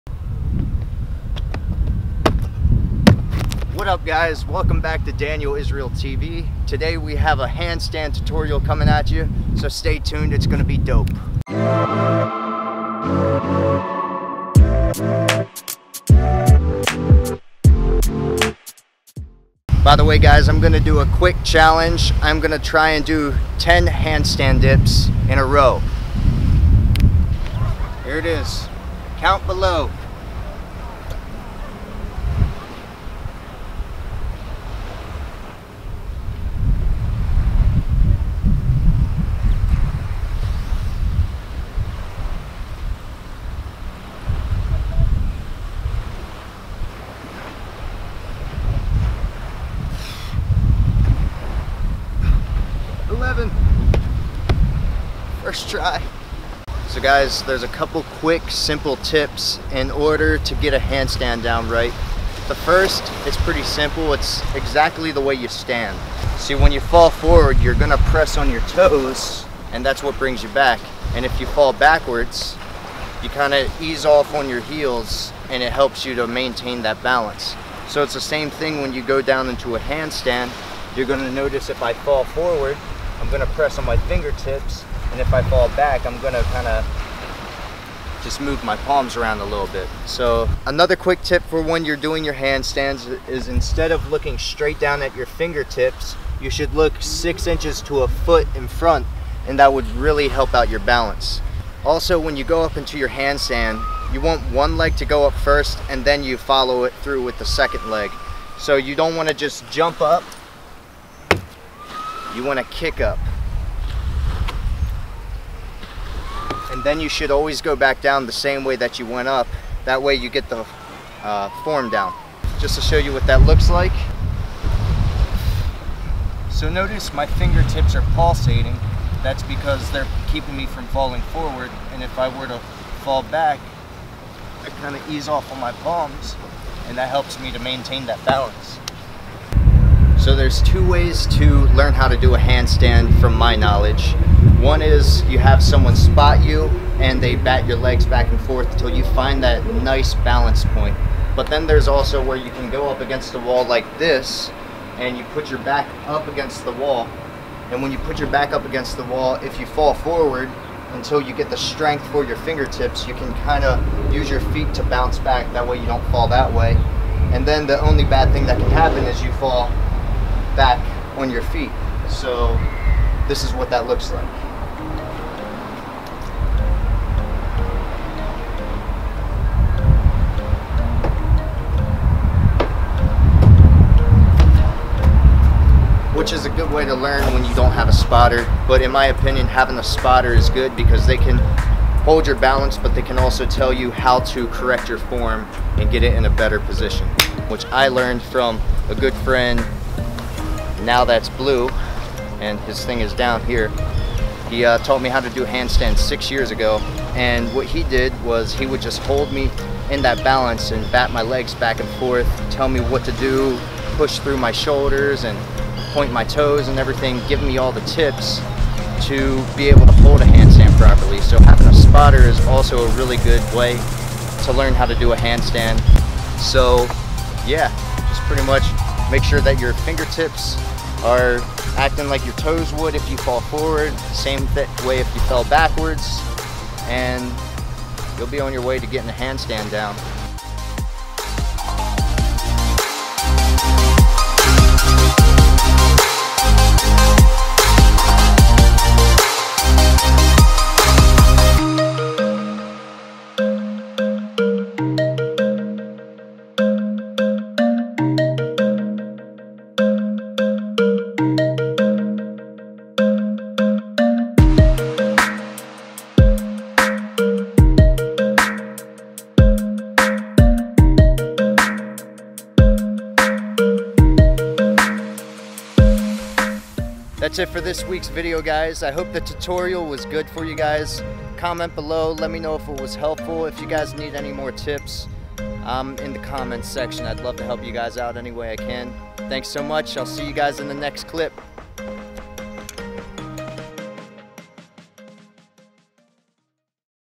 HOW TO STAND ON HANDS? |W/ HANDSTAND PUSHUPS!
I learned how to do a handstand 6 years ago by a buddy of mine. Check out his instagram
Instagram.com/Nowthatsblue
This is just a quick video of some pointers on how to hold a handstand properly. If you are in shape and your PRACTICE everyday you should have a handstand in a couple weeks. Hope the video works for you guys! Also you will se handstand push ups.

Handstand practice is great for your body and it is a really cool skill to learn, Not many people know how to to a handstand balance so I hope this video helps!